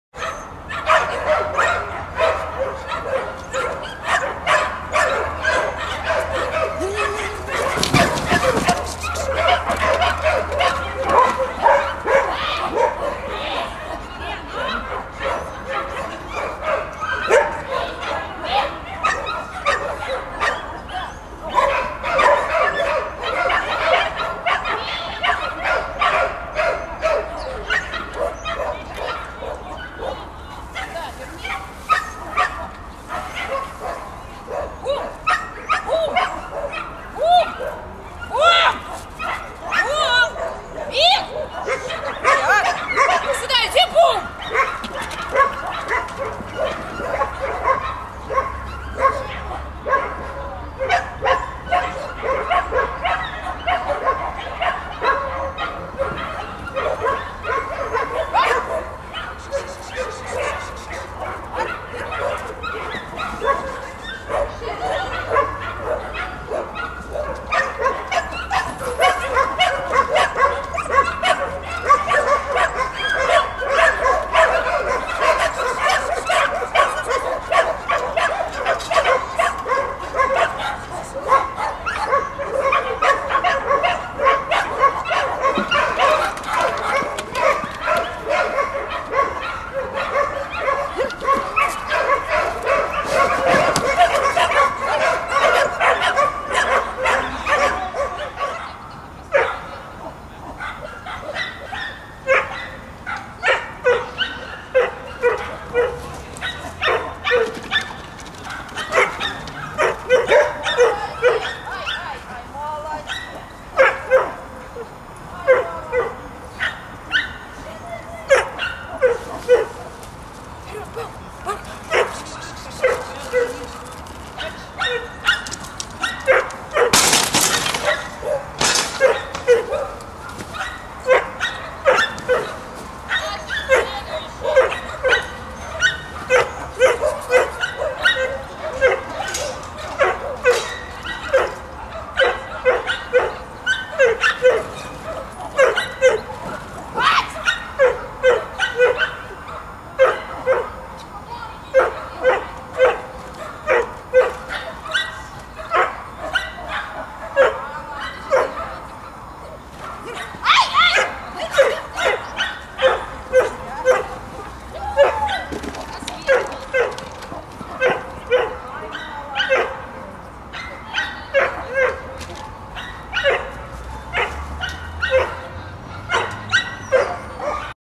Соревнования по аджилити 06.01.15 г. СПб, Пионерская v05
Любовь Тимина и бордер колли Мик.